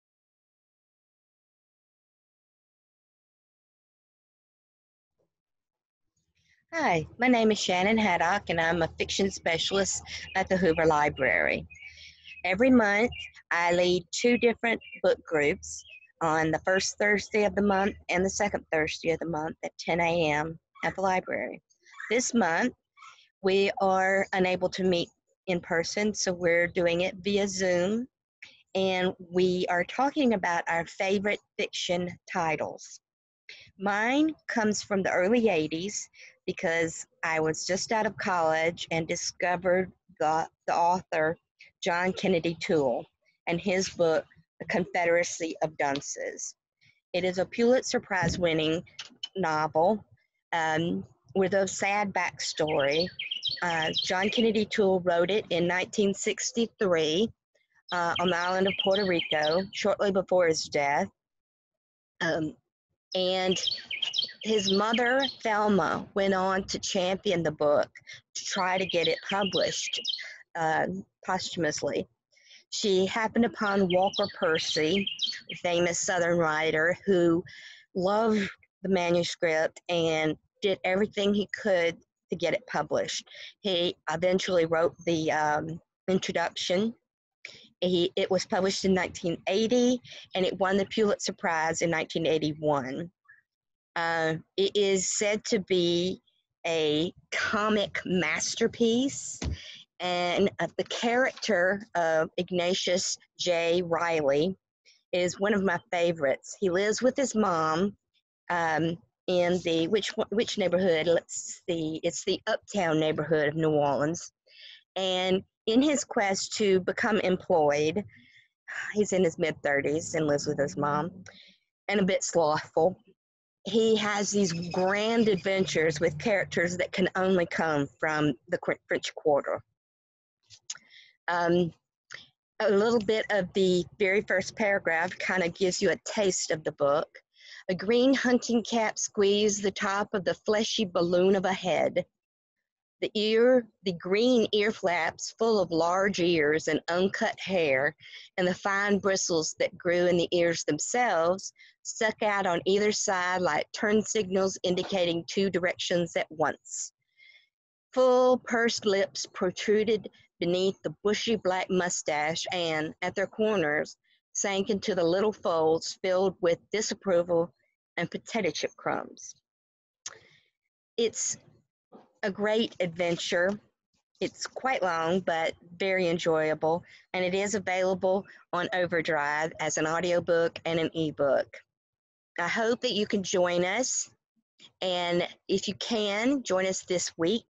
Readers Advisory: My Favorite Fiction
Join Shannon for another great Readers Advisory as she discusses her favotite fiction book A Confederacy of Dunces by John Kennedy Toole!

Don't forget to join her with your favorite fiction book during this month's Thursday Book Groups!